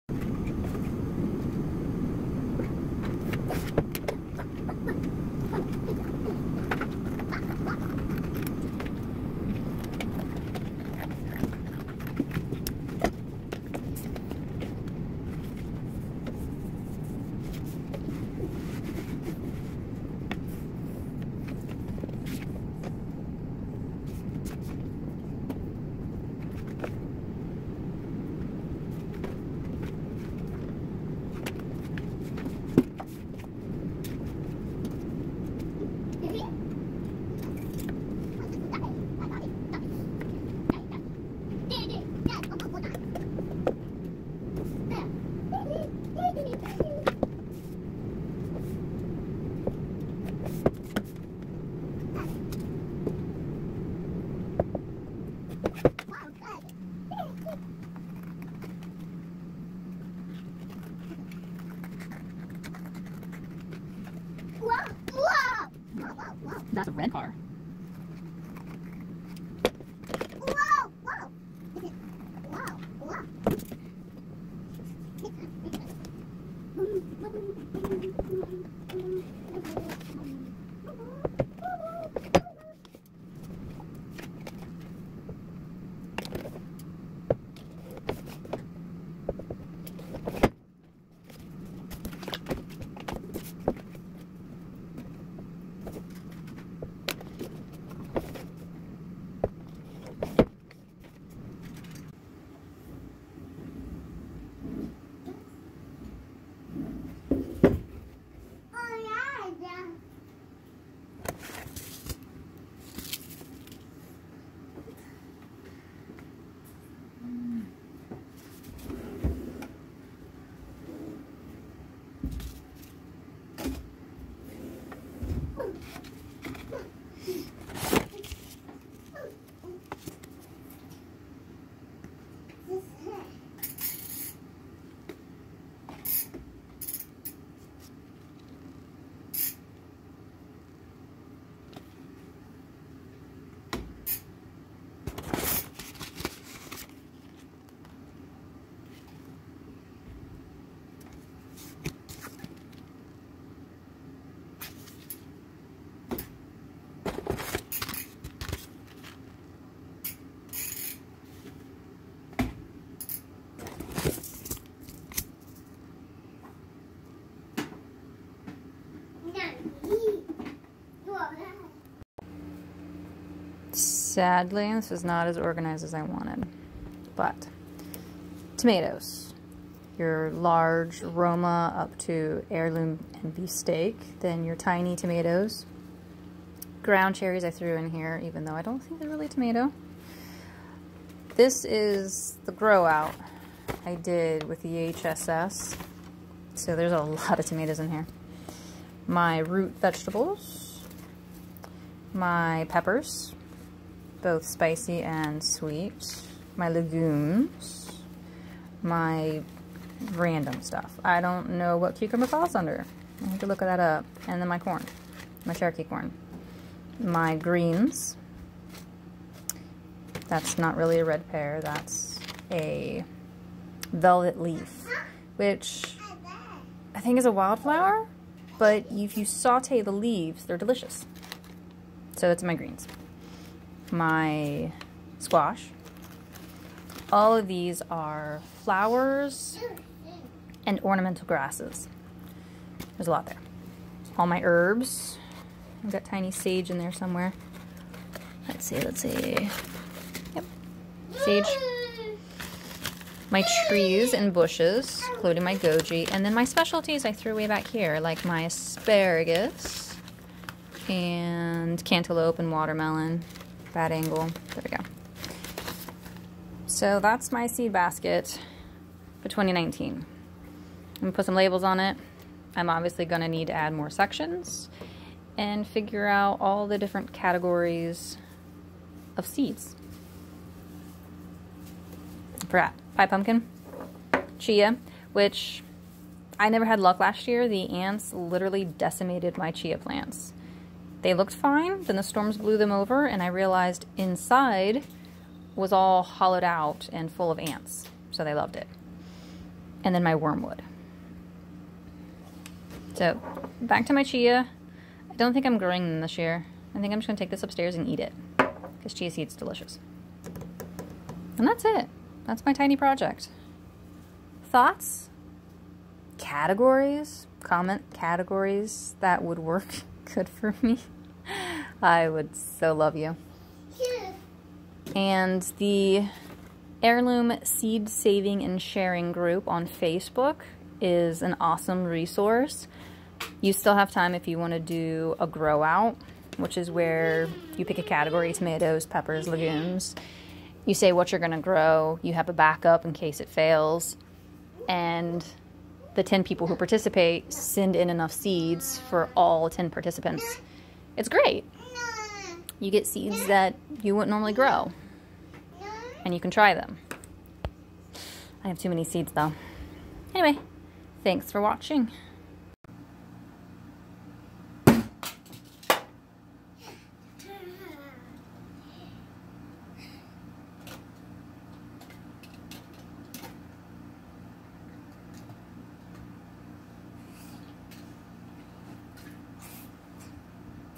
Organize Those Seeds & Chia Results
My mini project to organize my seeds & open a seed library in my town. It's step 1 of my community project which can be found at gf.me/u/xjkscr or my Instagram @patientfarmer

Also, results of my chia growing experience last year.